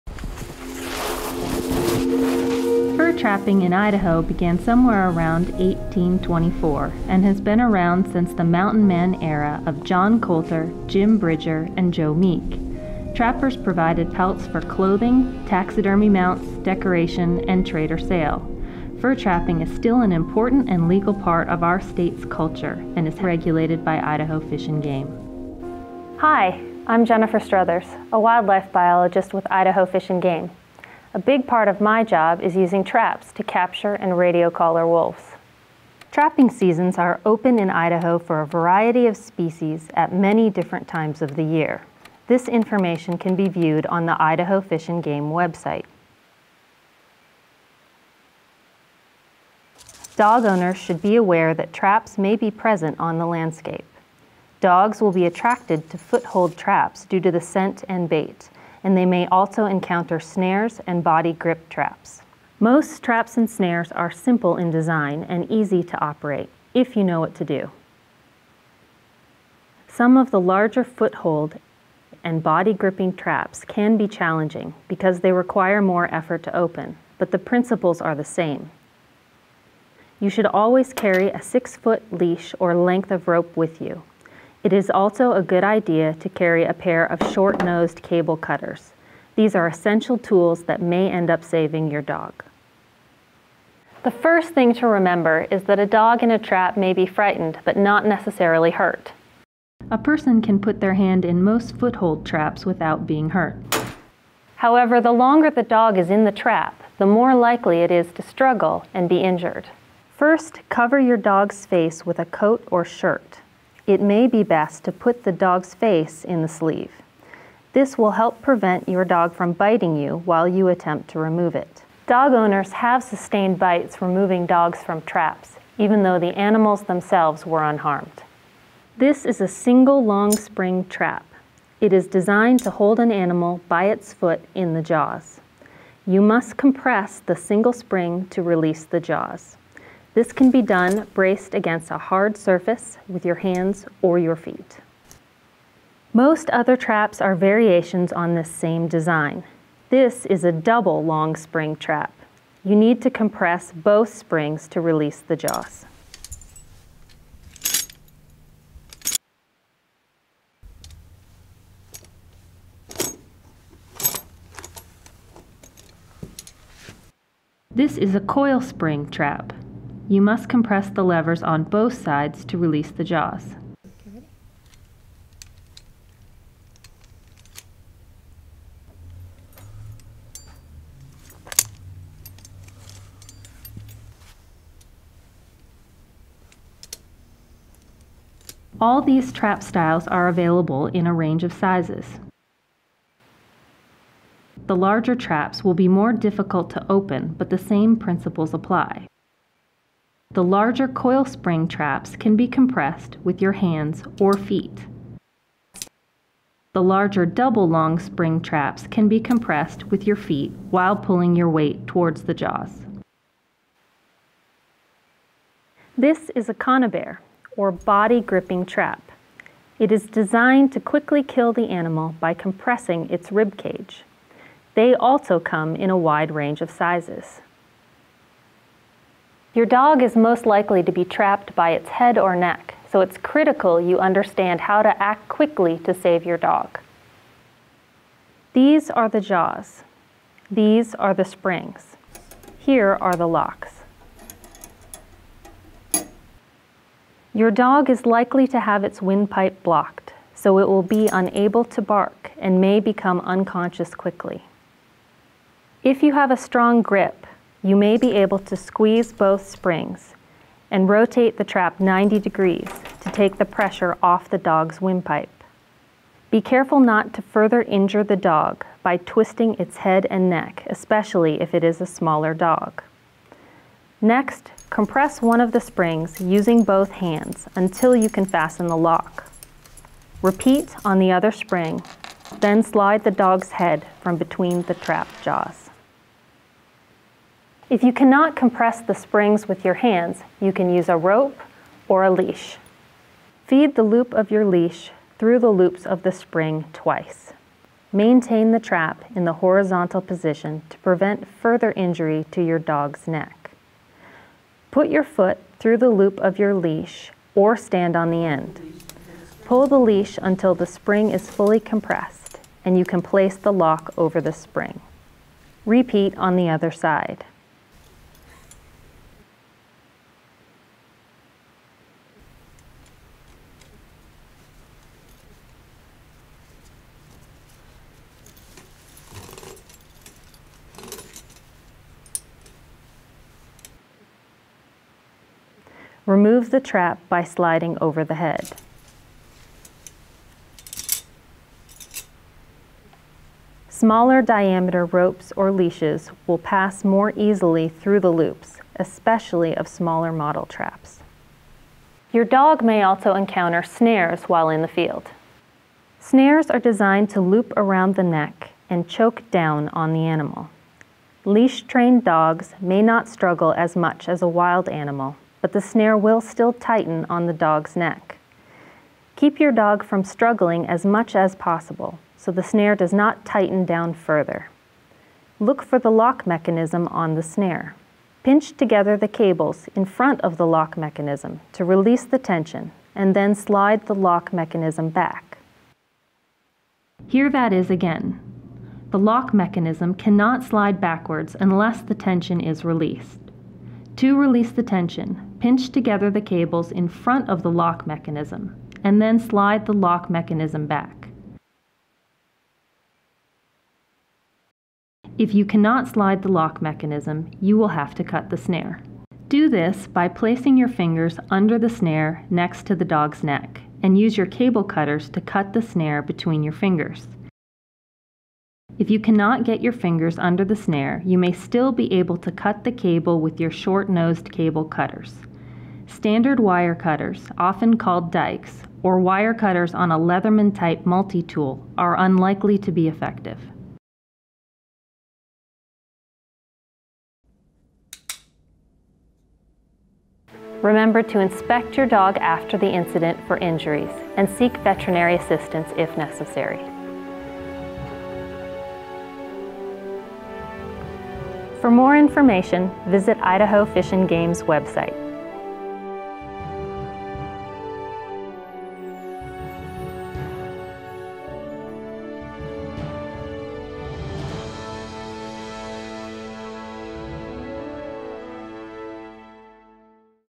Releasing your Dog from a Trap
Dog owners should be aware that traps may be present on the landscape. Dogs will be attracted to foothold trap sets due to the scent and bait, and they may also encounter snares and body grip traps. This is an instructional video explaining a variety of traps and how to release your dog from traps.